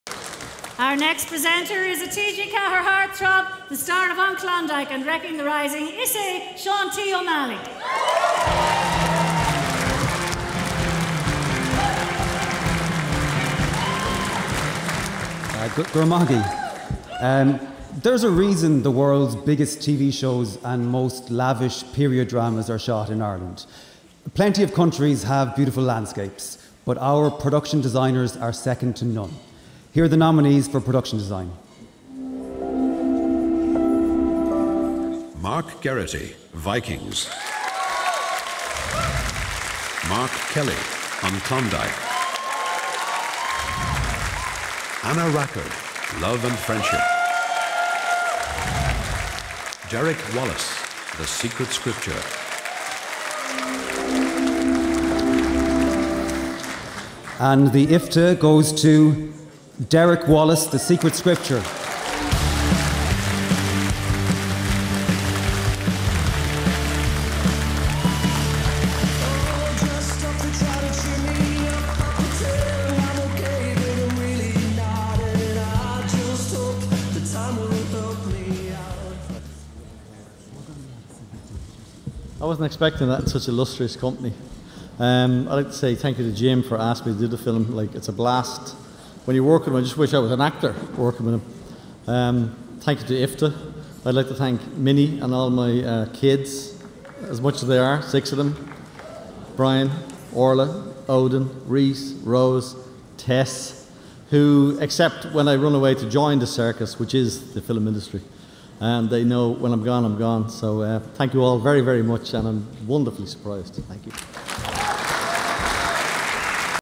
Derek Wallace -The Secret Scripture Winner Production Design 2017
Presented by Actor  Seán T. Ó Meallaigh to  Derek Wallace -The Secret Scripture Winner Production Design 2017